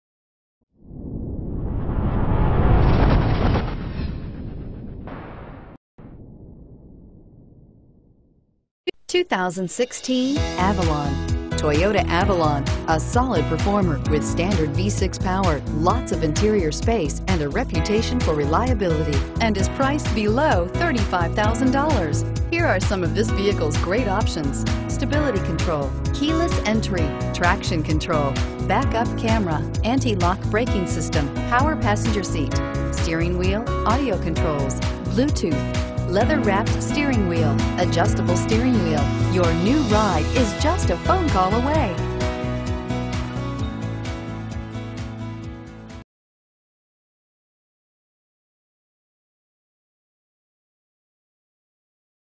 2016 Toyota Avalon Metairie LA GL3492
2016 Toyota Avalon XLE

Lakeside Toyota
3701 North Causeway

Looking for the right sedan? Today could be your lucky day. With 0 miles, this Midnight Black Metallic, 2016 Toyota Avalon equipped with Automatic transmission could be yours. Request more information and set up a test drive right away.